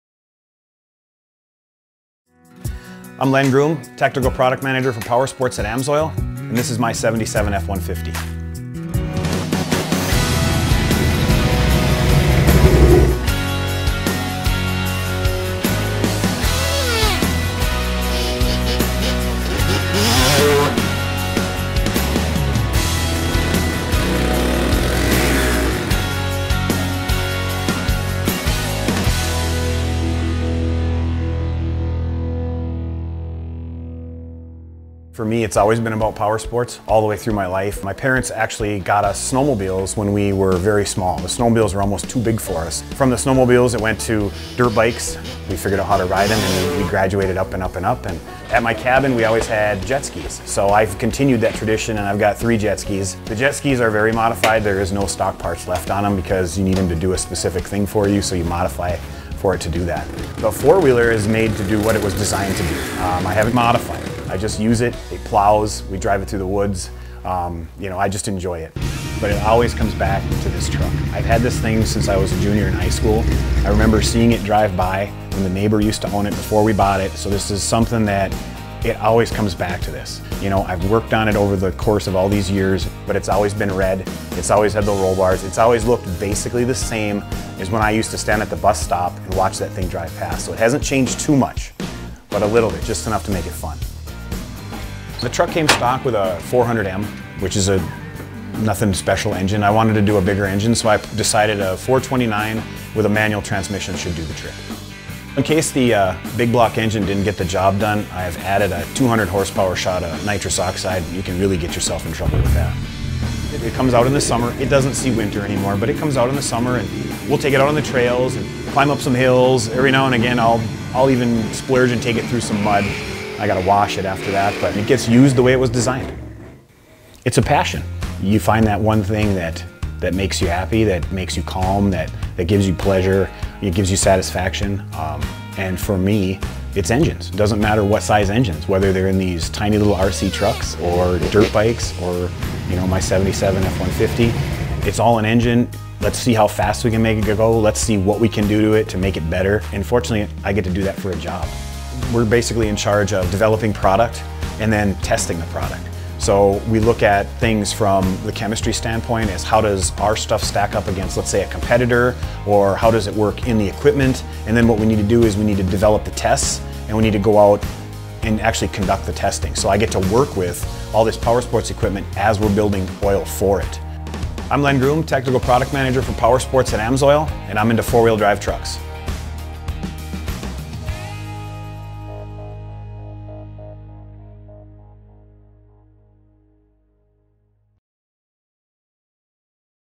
A Company of Enthusiasts: We're Into 4x4's
"It’s a passion. Your find that one thing that makes you happy, that makes you calm. It gives you satisfaction. For me, it’s engines.” Watch the video to see Len’s full story.